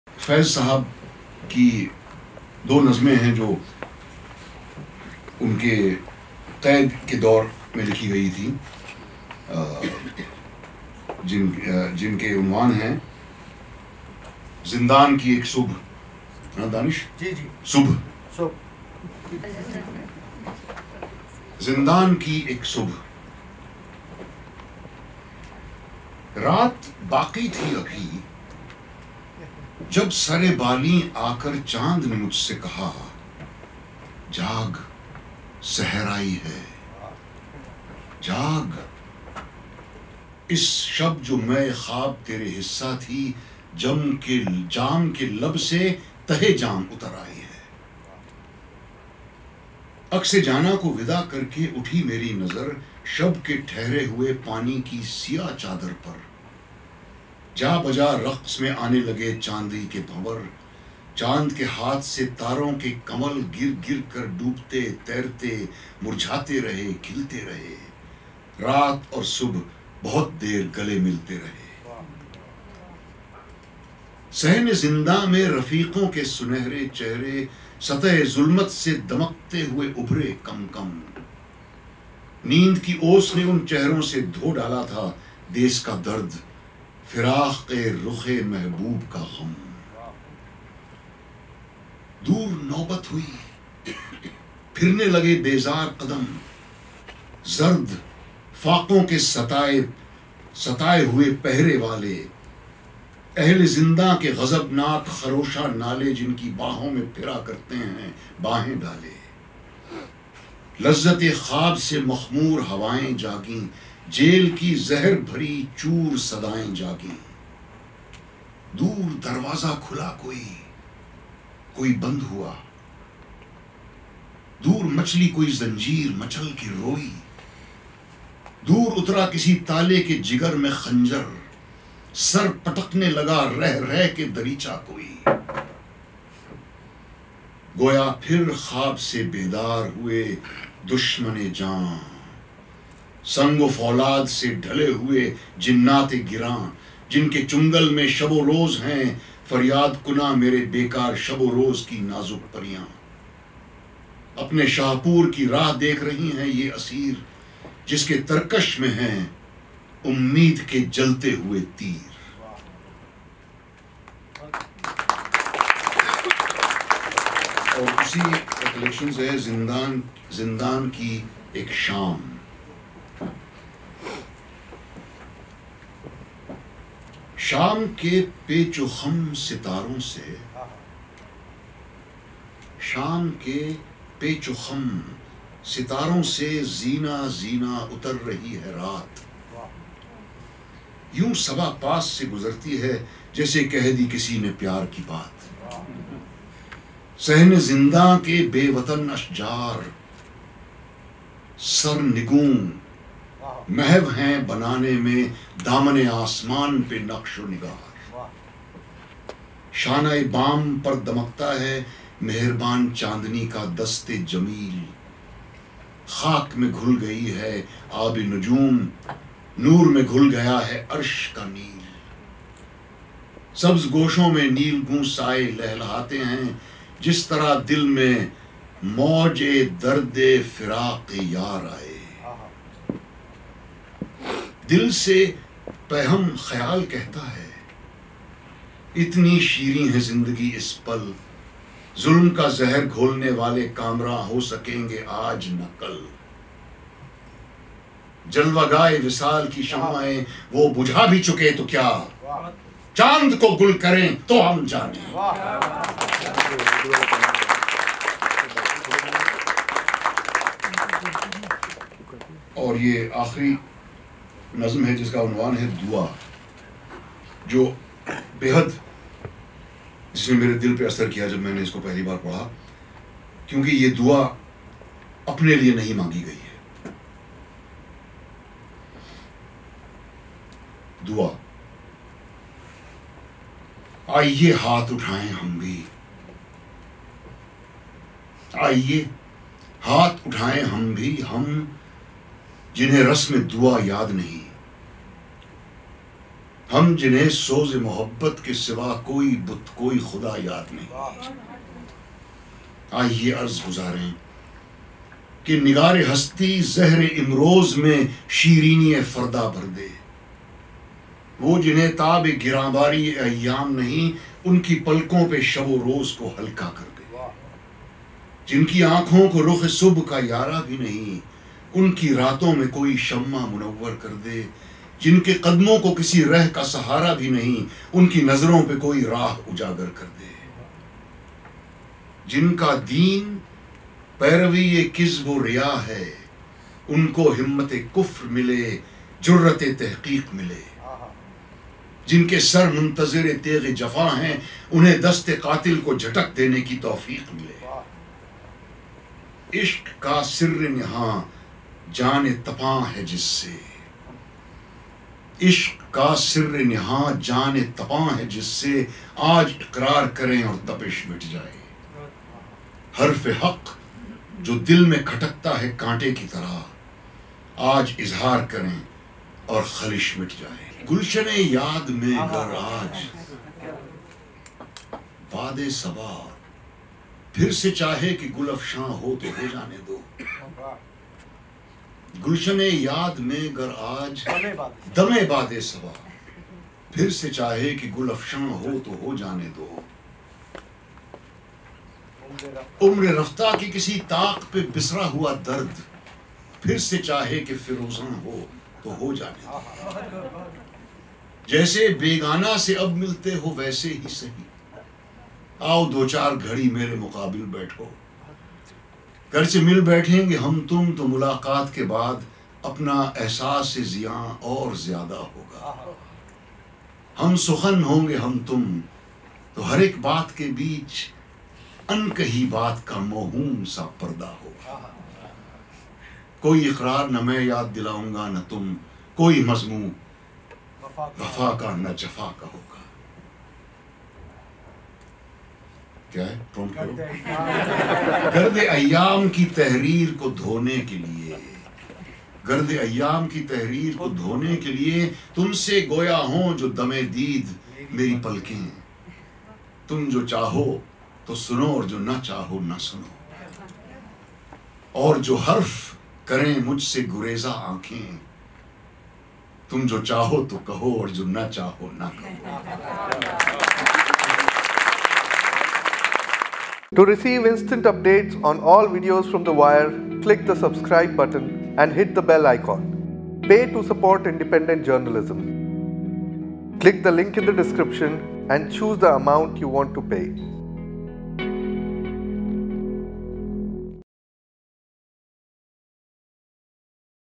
Naseeruddin Shah Reads Poems by Faiz Ahmad Faiz - PART 2
Veteran actor Naseeruddin Shah reads Faiz Ahmad Faiz's poems at The Wire Urdu's anniversary event in Mumbai on August 18, 2018.

This is a recording of a discussion that was held in Mumbai at the G5A Foundation for Contemporary Culture.